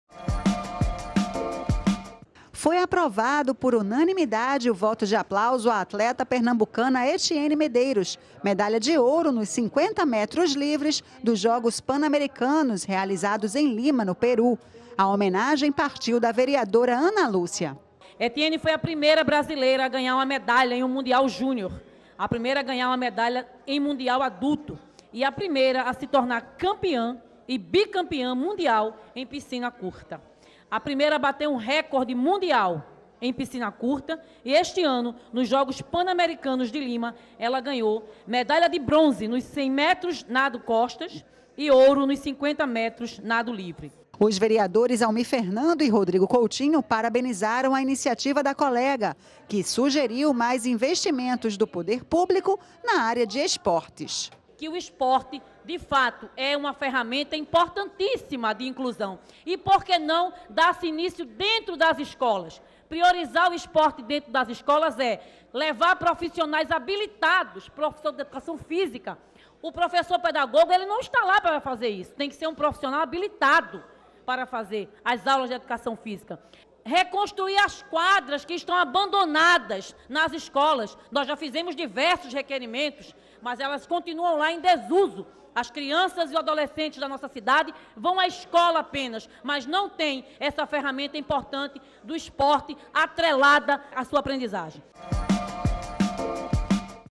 Câmara concede voto de aplauso a Etiene Medeiros
Foi aprovado por unanimidade o voto de aplauso à atleta pernambucana Etiene Medeiros, medalha de ouro nos 50 metros livre dos jogos Pan-Americanos realizados em lima, no Peru. A homenagem partiu da vereadora Ana Lúcia.